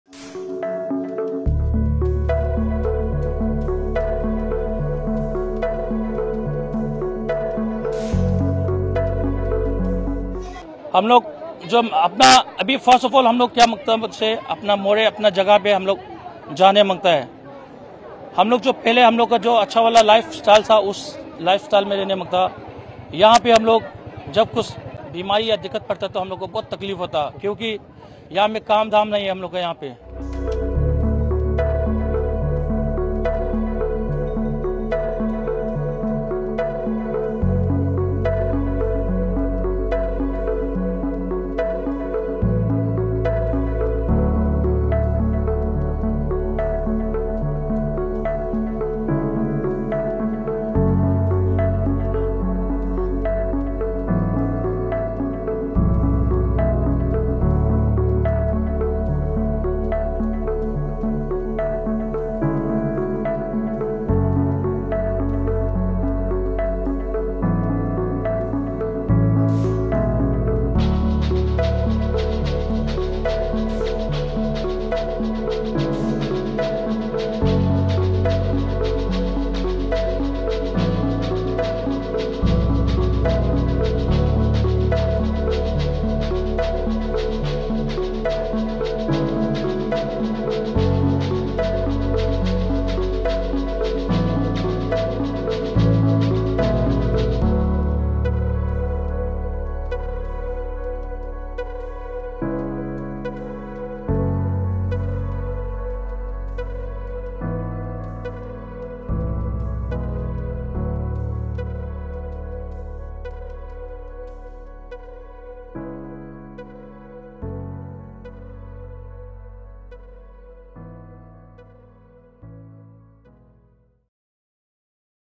Fresh protest rocks Imphal as people displaced by violence raise voice, police fire tear gas
Fresh protest rocks Imphal as people displaced by violence raise voice, police fire tear ga

A massive rally was taken out by people displaced by violence in Manipur’s Imphal. Unhappy by the state government, the protesters marched on streets to raise their concerns. A large contingent of police and CRPF was deployed. The security forces fired tear gas shells as protest turned violent.